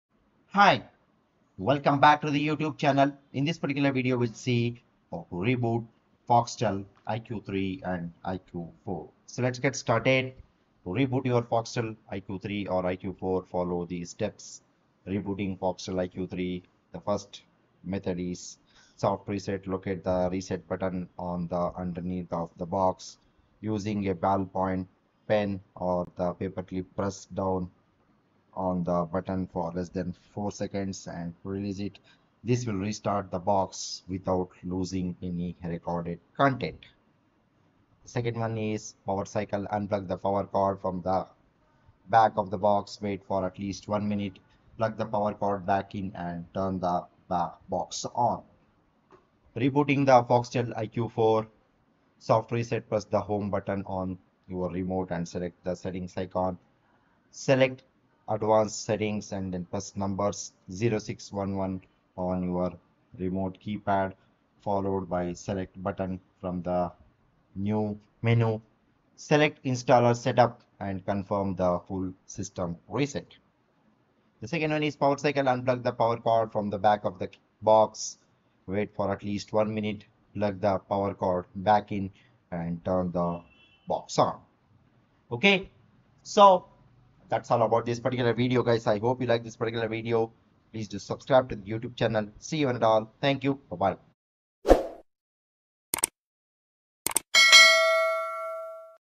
How To Reboot Foxtel Iq3 & Iq4
Today, we're going to show you How To Reboot Foxtel iq3 & iq4.This can be helpful in various situations, such as Rebooting Foxtel iQ3 and iQ4 ensures improved performance, resolves technical issues, enhances viewing experience, and maintains stable, reliable service.